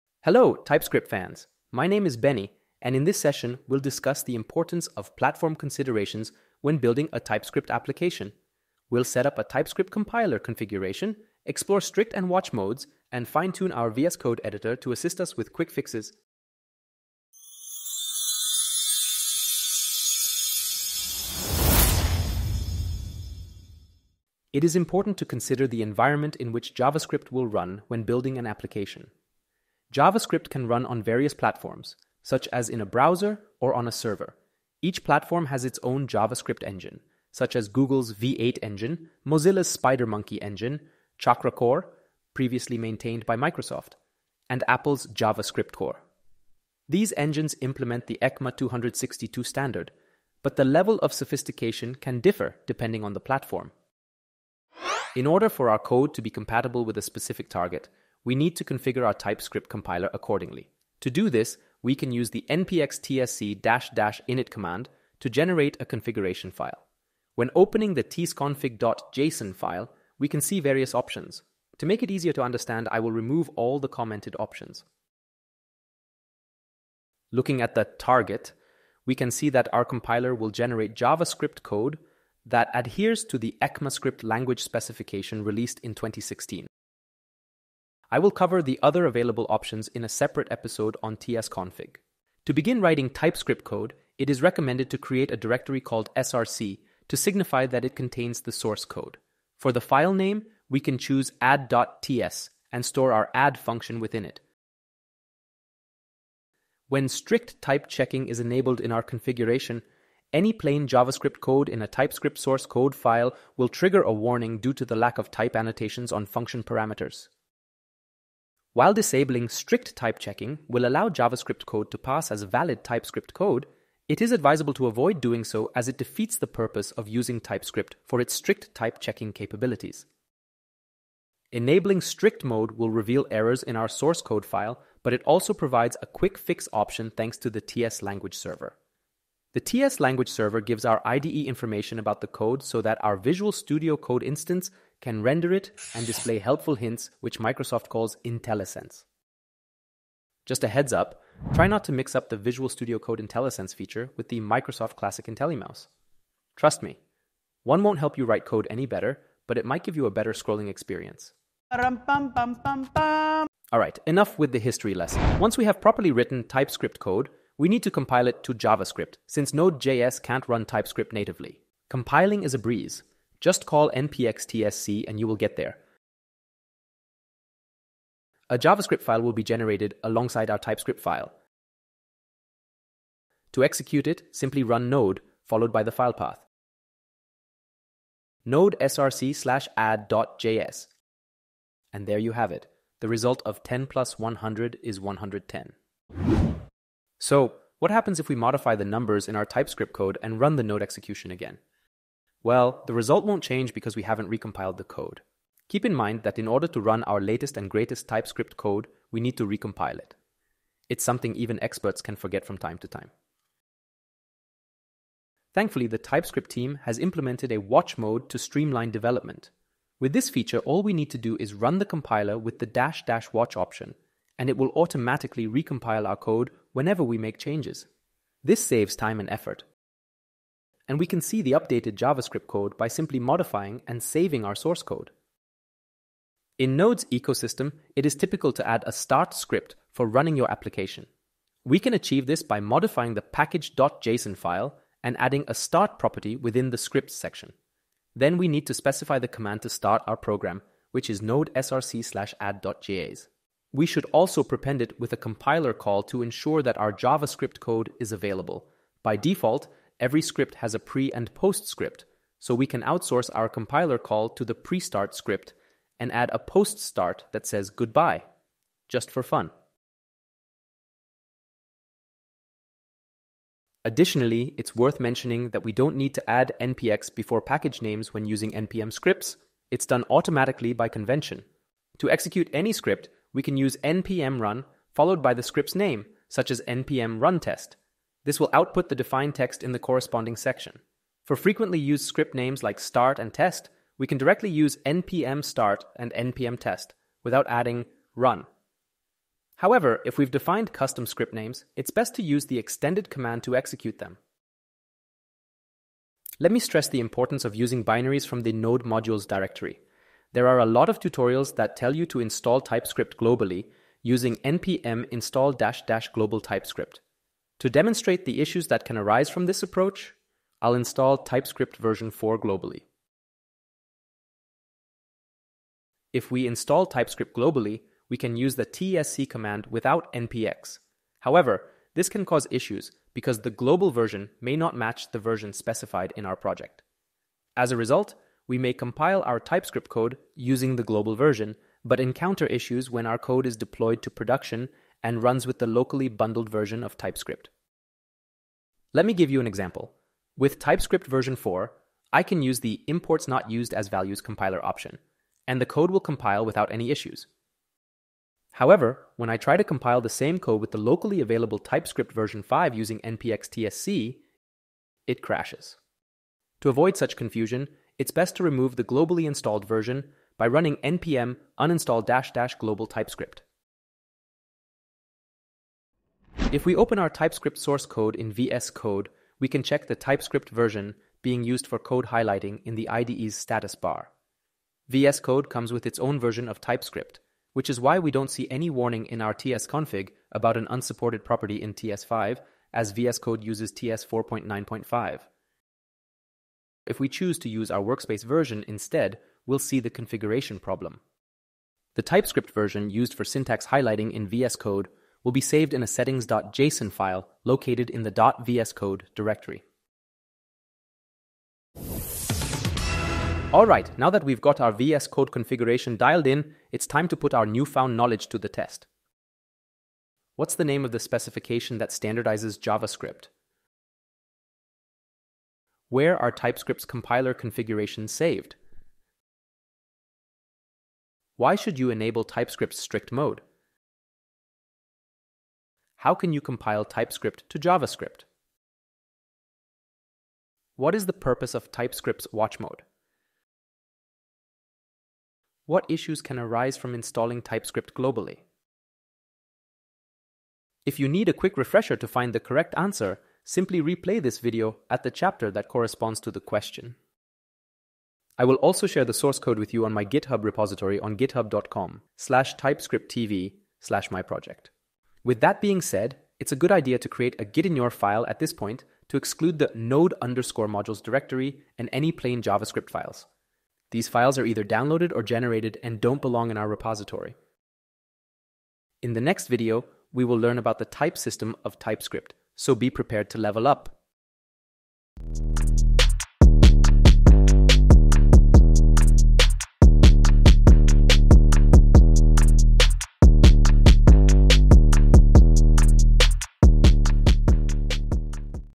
TypeScript Compiler (tsc) configuration in VS Code
In this episode, we explore the importance of considering the platform for our TypeScript application. We also discuss the --watch option to streamline development, and caution against the potential issues of installing TypeScript globally.

Timeline:
0:00 Introduction
0:24 Runtime Environments & JavaScript Engines
0:55 Setting Up TypeScript Compiler Configuration
1:47 Enabling Strict Type Checking
2:19 Quick fixes with Visual Studio Code IntelliSense
2:52 Compiling TypeScript into JavaScript
3:35 Using TypeScript's Watch Mode
3:56 Introduction to npm Scripts
5:06 Executing Binaries from Node Modules
5:48 Issues with Global TypeScript Installations
6:16 Specifying VS Code's TypeScript Version
6:52 Quiz Time
7:58 Preview of Upcoming Video